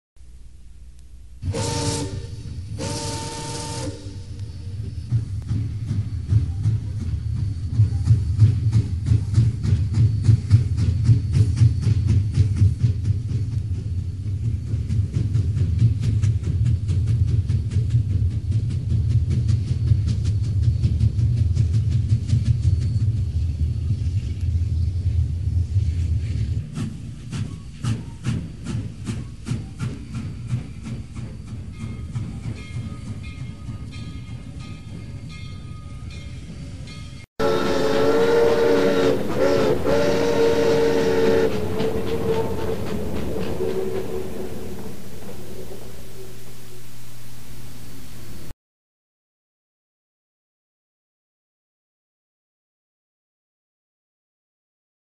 Blue Comet Coming into Point Pleasant '75
This is another single still slide I found of the Blue Comet. I re-cropped to mimick motion and increase detail